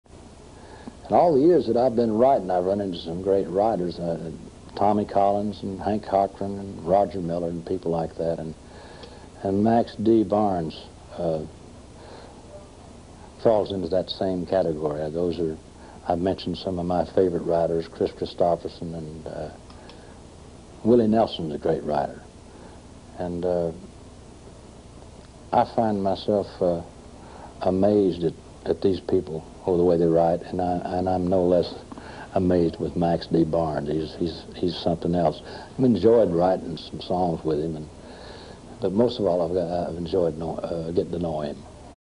Merle Haggard talks about Max D. Barnes
This is an interview with Merle Haggard circa 1993 in Which Merle praises two time CMA song of the year winner and Hall of fame member Max D. Barnes